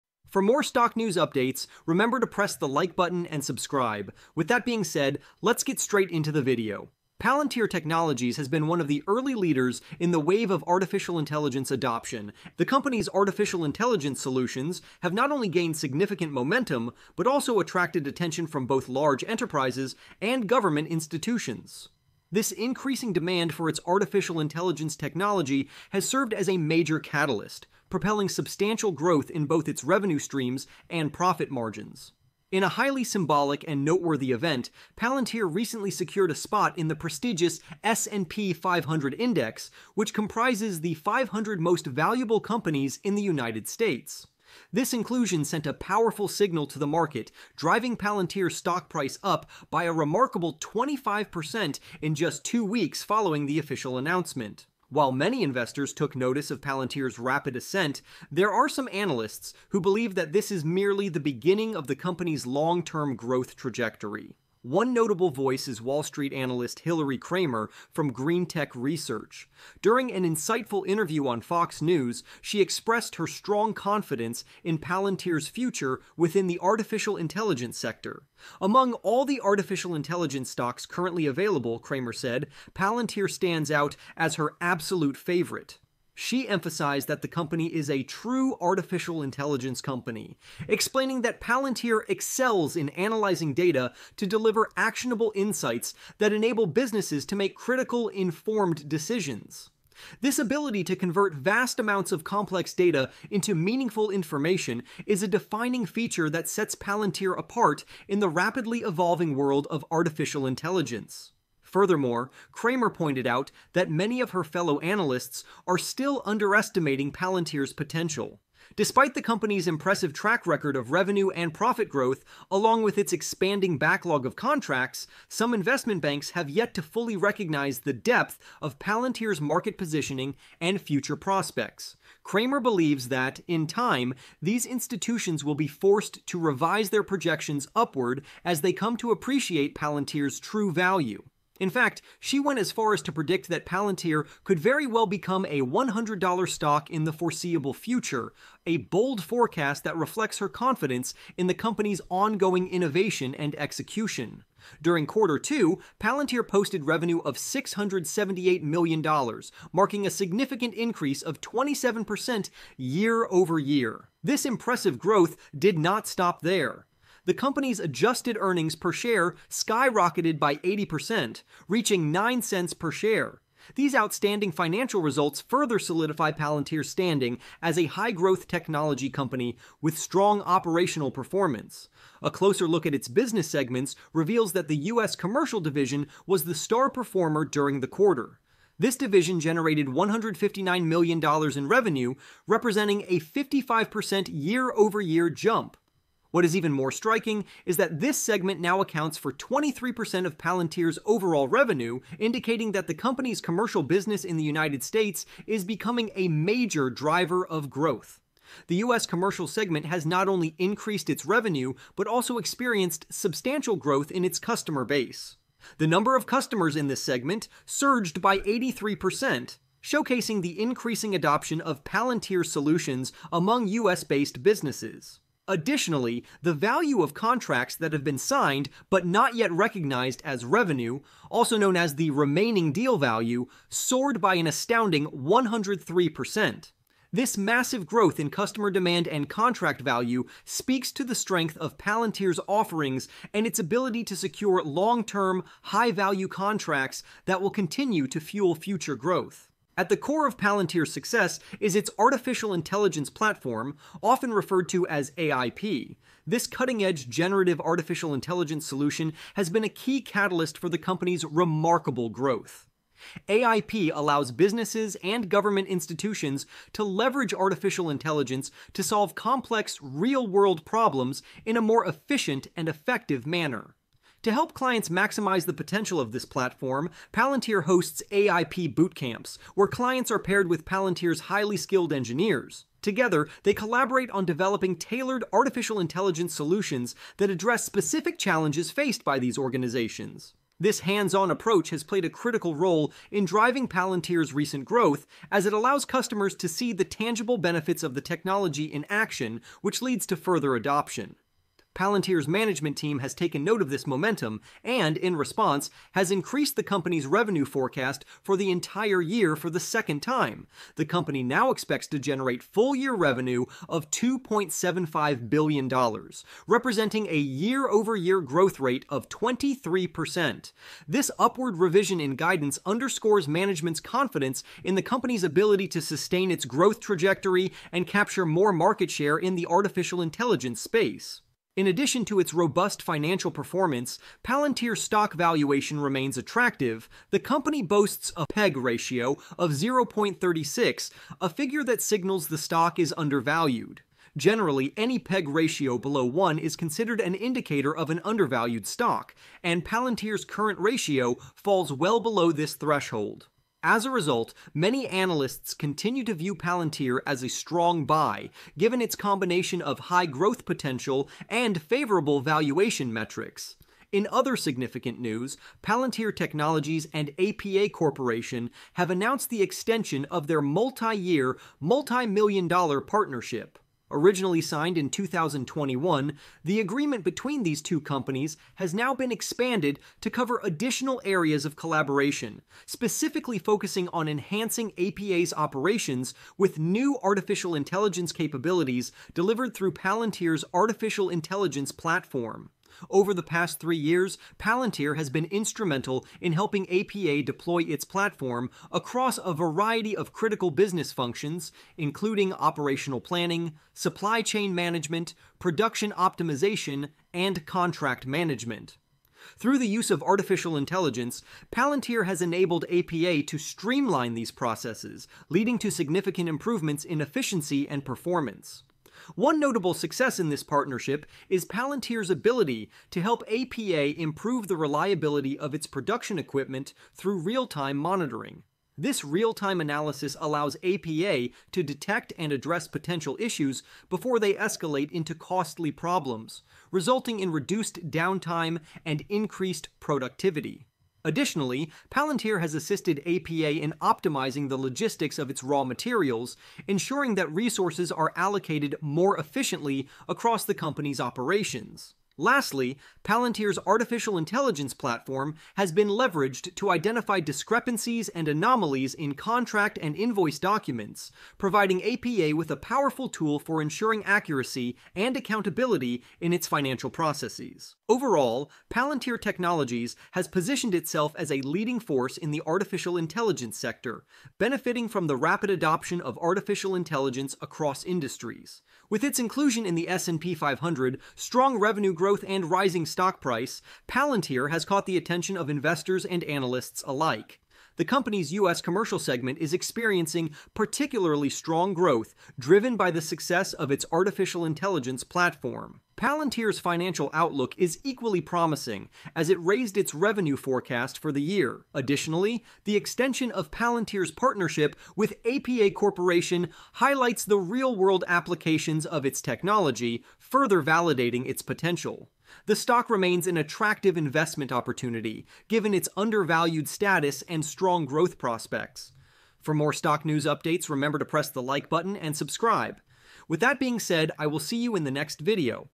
Palantir to $100: Breaking Down the Bullish Case for This AI Stock!
You won’t believe what’s going on with Palantir! This AI powerhouse just joined the S&P 500, and its stock has jumped twenty-five percent! Analysts are buzzing about its future, calling it a potential hundred-dollar stock. From massive revenue growth to its game-changing AI platform, Palantir is crushing it! The U.S. commercial sector is booming, and they just scored a major partnership extension with APA Corporation! Get hyped, because this AI stock is going places!